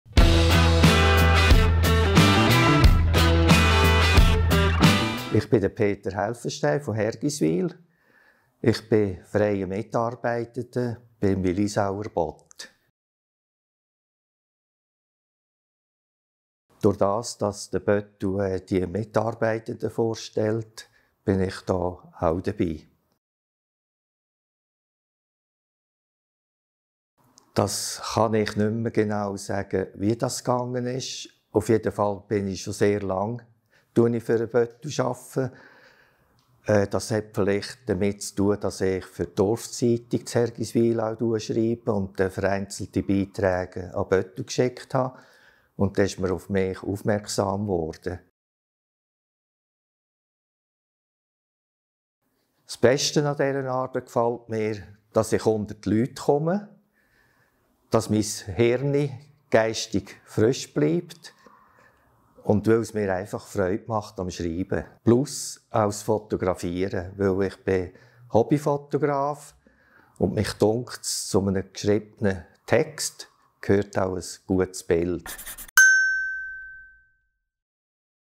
Grüezi | Peter Helfenstein | Ausblick auf den «Böttu» vom 22. Mai 2024
Der «Böttu» darf bei der Berichterstattung auf die zuverlässige Hilfe von freien Mitarbeiter/innen zählen. Peter Helfenstein ist einer von ihnen. Er gibt einen kurzen Ausblick auf den Bericht in der Ausgabe vom 22. Mai.